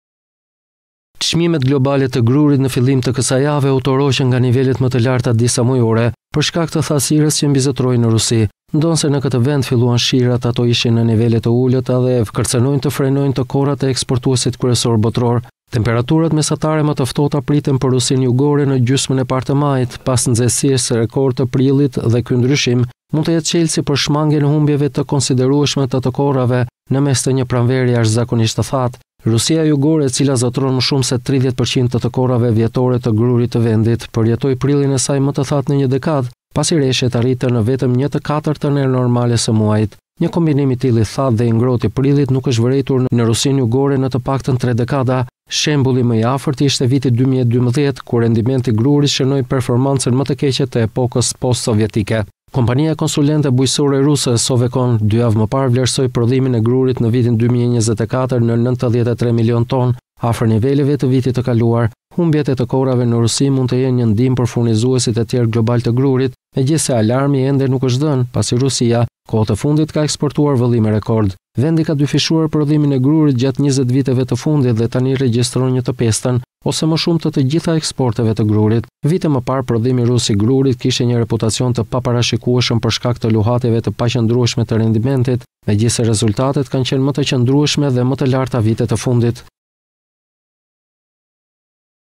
Prodhimi i grurit rus, në pikëpyetje – Shkak, temperaturat e larta dhe thatësira e prillit
Çmimet globale të grurit në fillim të kësaj jave u tërhoqën nga nivelet më të larta disamujore për shkak të thatësirës që mbizotëroi në Rusi. Ndonëse, në këtë vend filluan shirat, ato ishin në nivel të ulët dhe kërcënojnë të frenojnë të korrat e eksportuesit kryesor.